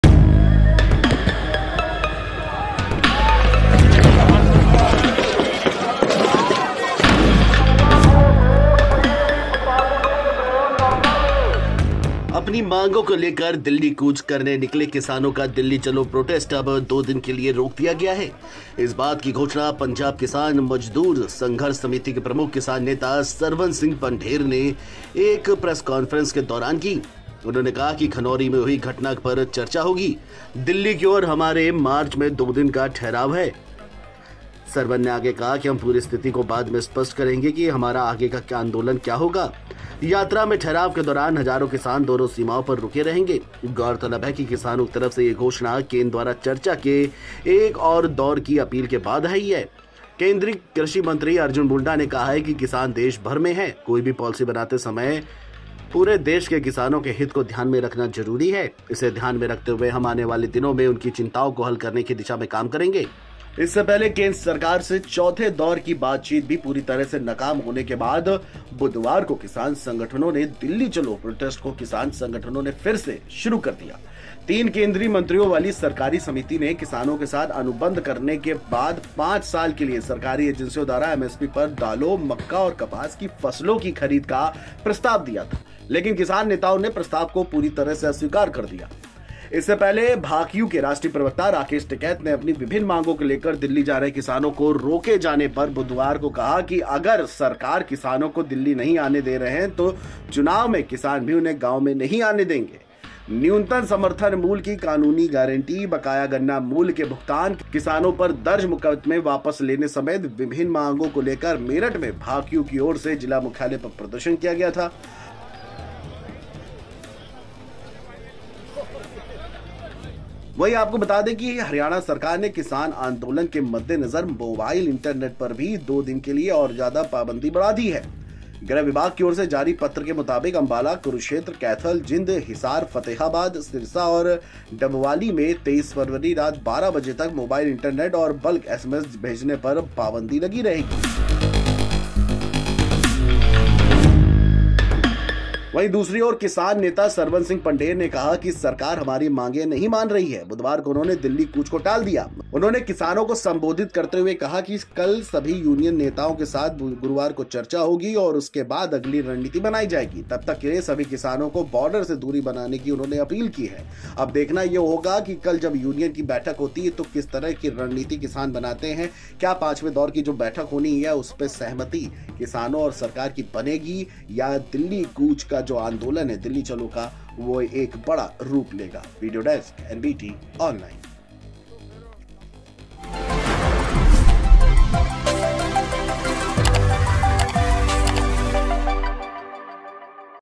Farmers Protest 2024: जानिए आखिरी क्यों थम गया किसानों का दिल्ली चलो आंदोलन? | NBT
अपनी मांगों को लेकर दिल्ली कूच करने निकले किसानों का ‘दिल्ली चलो’ प्रोटेस्ट दो दिन के लिए रोक दिया गया है। इस बात की घोषणा पंजाब किसान मजदूर संघर्ष समिति के प्रमुख किसान नेता सरवन सिंह पंढेर ने एक संवाददाता सम्मेलन में बुधवार को की। उन्होंने कहाकि खनौरी में हुई घटना पर चर्चा होगी। दिल्ली की ओर हमारे मार्च में दो दिन का ठहराव रहेगा। सरवन ने आगे कहाकि हम पूरी स्थिति को बाद में स्पष्ट करेंगे कि हमारा आगे का आंदोलन क्या होगा। यात्रा में ठहराव के दौरान हजारों किसान दोनों सीमाओं पर रुके रहेंगे।